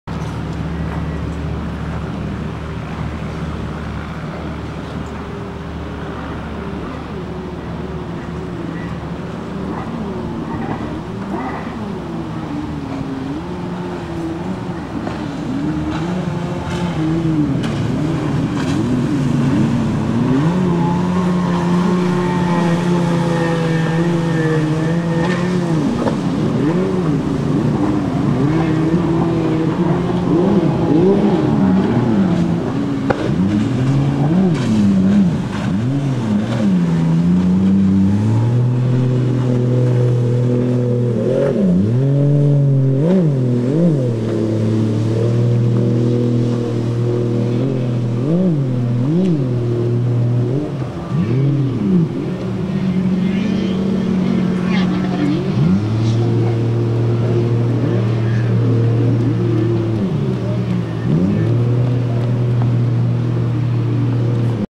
Ship to Sea, Miami Florida USA
Ship to Sea, Miami Florida USA Goverment cut , Miami, FLA 11-21-10  Pilot boat, Jet skies, Shipping to the caribbean,